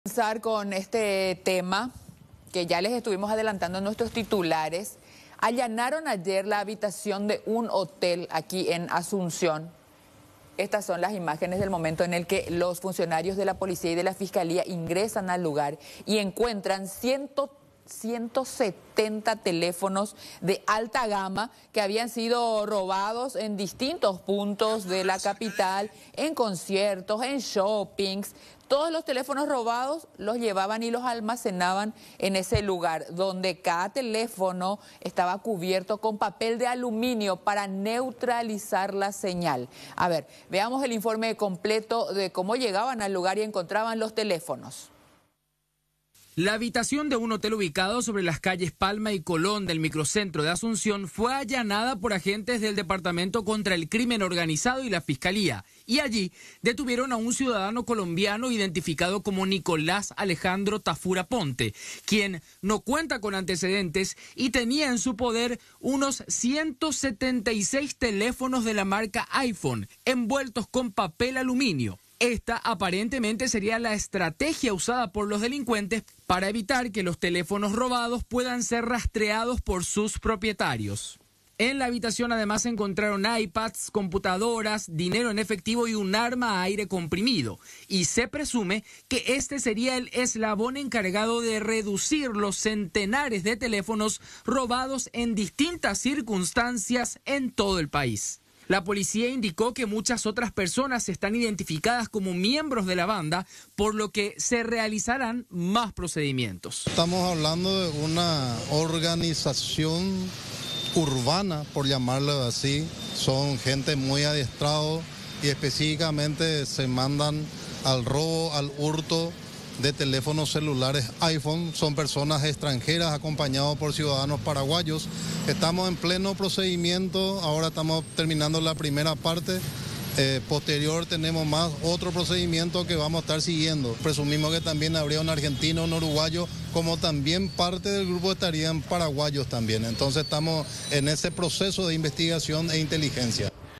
Recuperan celulares robados de alta gama tras allanamiento
Agentes de la Policía Nacional y del Ministerio Público allanaron una de las habitaciones de un hotel y encontraron más de 100 celulares robados de alta gama, en Asunción. Los aparatos se encuentran en el Departamento Contra el Crimen Organizado.